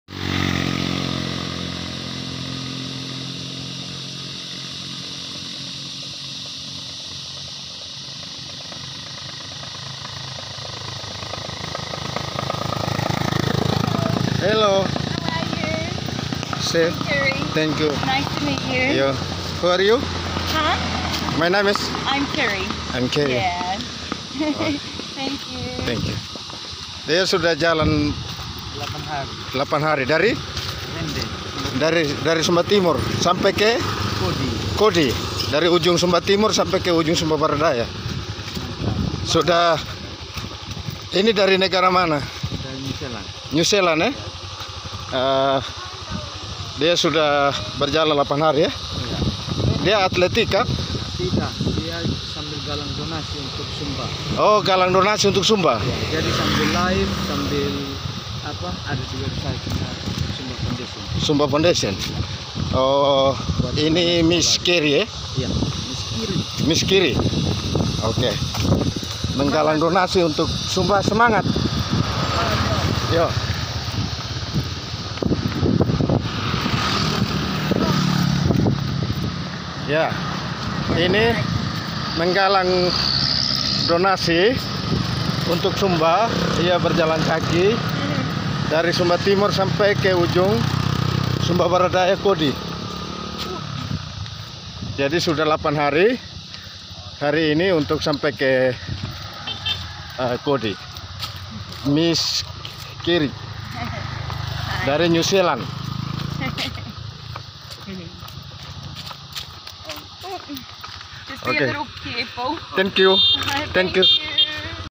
"KELILING SUMBA"BERJALAN KAKI DARI WAINGAPU KE KODI SBD.WARGA NEW ZEALAND.
Video dari Paulus Malo